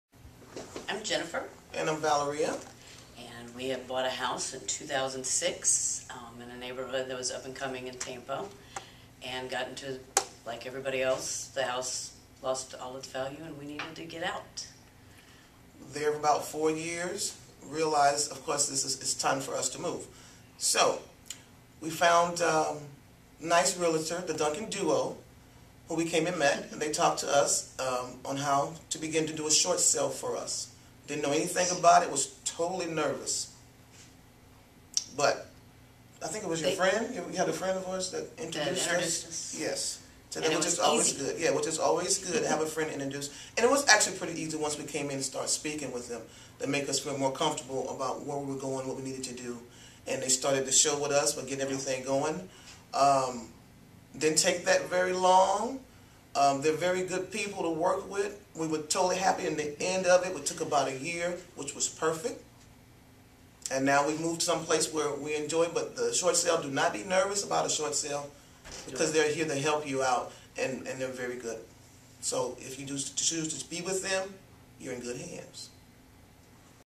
Duncan Duo Realtors helped Jennifer & Valeria short sale and avoid foreclosure in Tampa
Jennifer and Valeria speak on video about their experience with buying a home at the peak of the market in 2006 and needing help selling through a short sale.  The Duncan Duo helped explain things to make it easy for them and help them feel more comfortable about the process.  They were happy with the experience.  See more of our raving fans on our YouTube